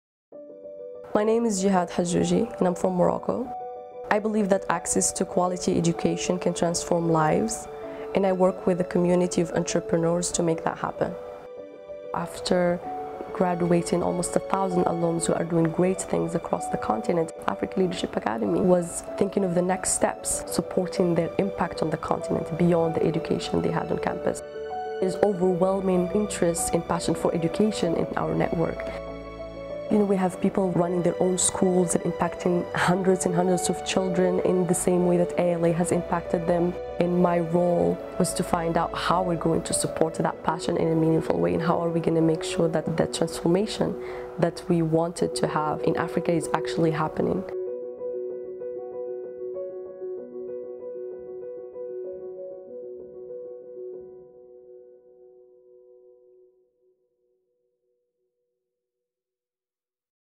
Jihad Hajjouji '08 - Redefine Expectations (Trailer)
Jihad Hajjouji '08 from Morocco leads a network of education entrepreneurs building high impact institutions across Africa through the Anzisha Education Accelerator. Drawing expertise from her earlier career as a Consultant with Dalberg Global Development Advisors, and learnings from Stanford University where she earned an MBA and Master's Degree in Education, Jihad seeks to transform the education landscape in Africa by empowering the next generation of education leaders.

Watch the full length feature on Jihad as part of African Leadership Academy's Redefine Expectation series